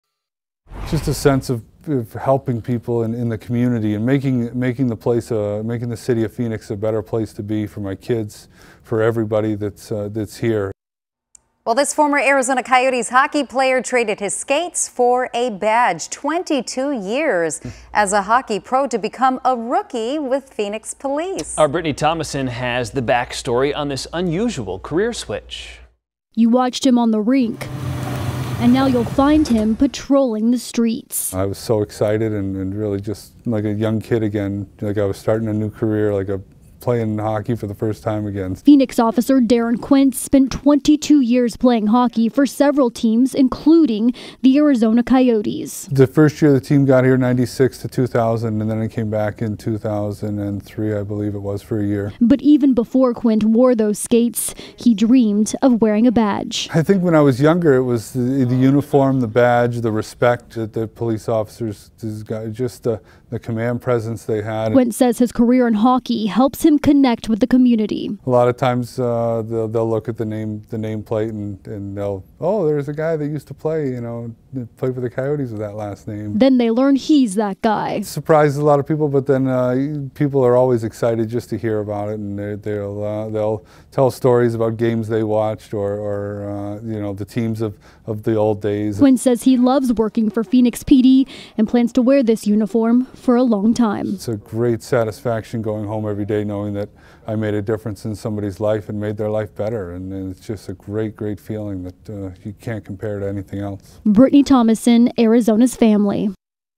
Former Arizona Coyotes hockey player joins Phoenix Police Department
A former Arizona Coyotes hockey player has traded his skates for a badge. After 22 years playing defense for teams across the world, Deron Quint became a Phoenix police officer last year.